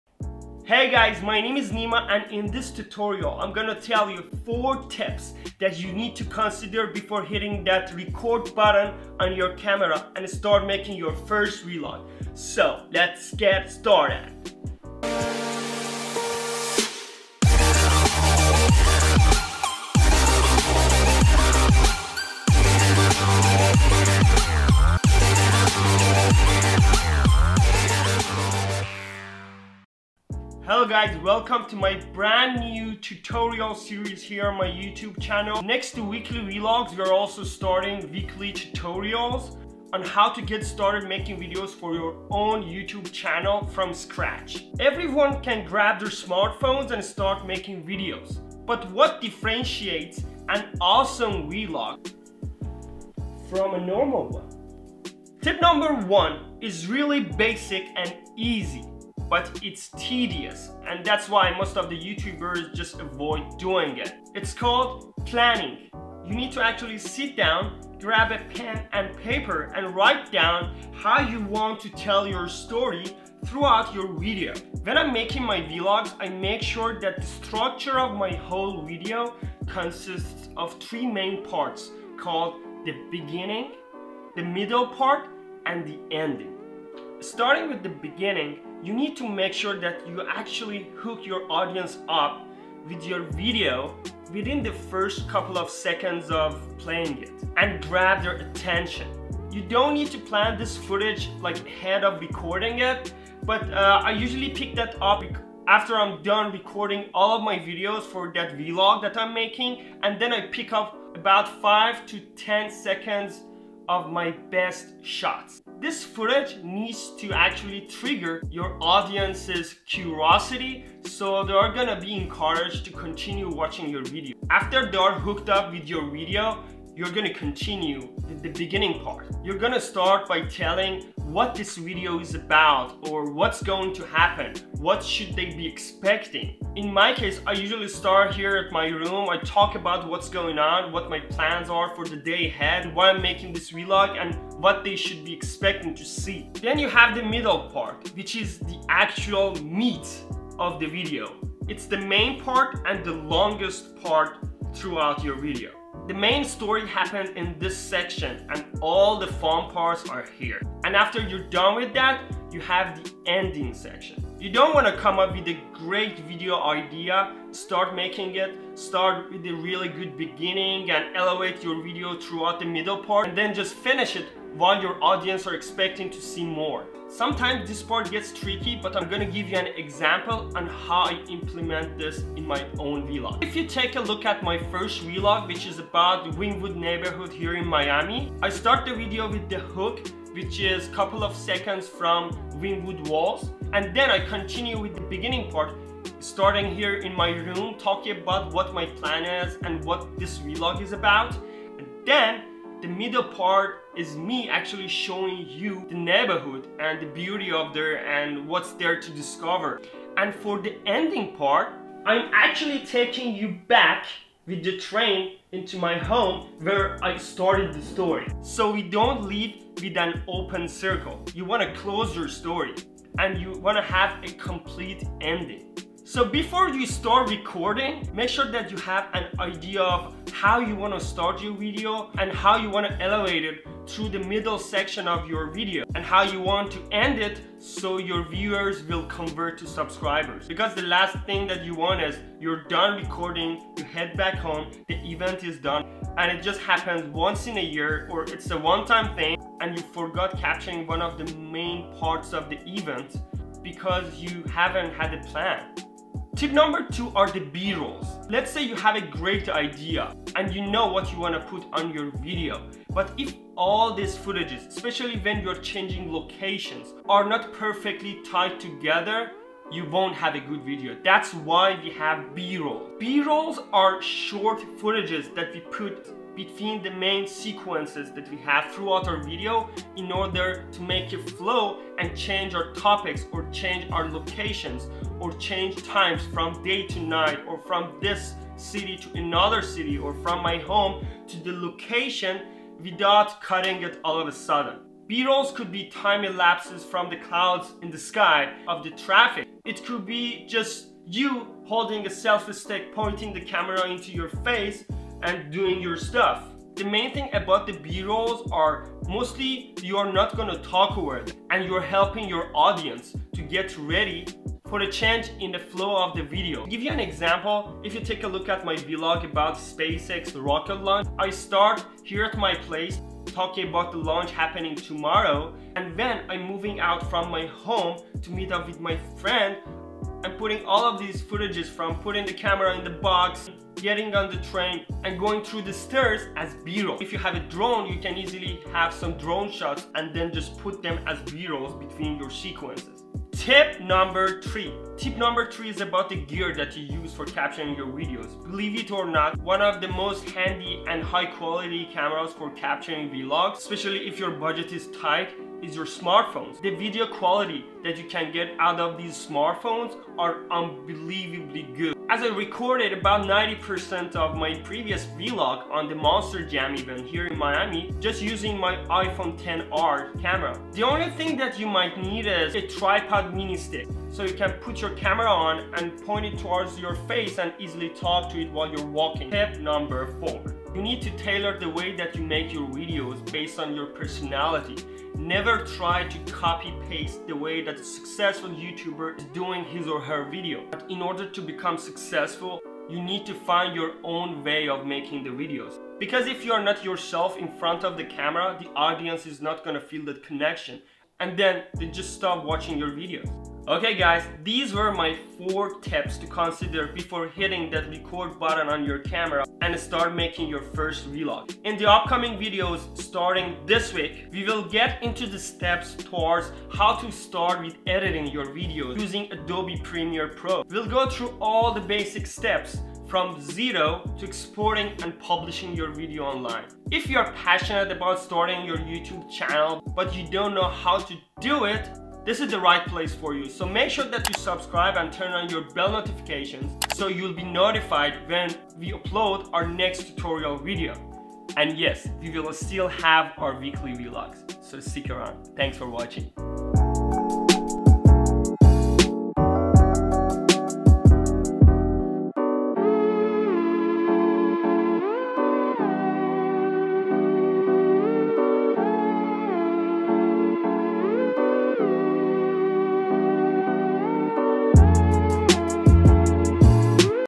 How To Get Started On Making Your First Vlog | Tutorial Series | Episode 1 | Persian Sub
Good News! I'm starting a new YouTube series apart from the weekly vlogs and I'm going to teach everyone who is interested in starting their own YouTube channel, how go get started on making their very first vlog in an easy and practical way! Starting with very basic steps which you need to consider before getting started on the recording process including planning your video!
I will be explaining my 4 tips that everyone should know starting this journey and in the upcoming episodes, we'll talk about video editing from scratch using Adobe Premier Pro ! covering basic editing, color corrections, audio adjustment and everything you need to know in order to make a successful YouTube Video!